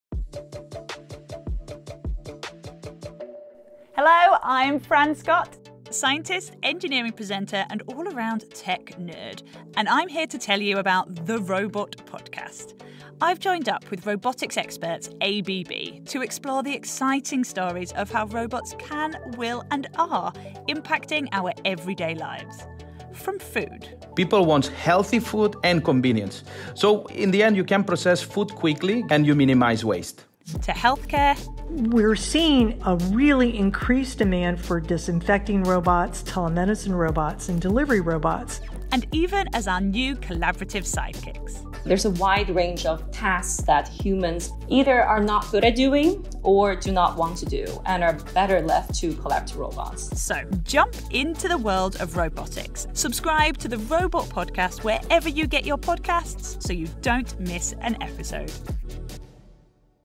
The Robot Podcast
The Robot Podcast: In this series, we’re exploring the innovative and exciting world of robotics and the ways in which it benefits society. In each episode, our host Fran Scott leads an expert panel discussion, delves into international case studies and explains how automation is changing the world of the future, today.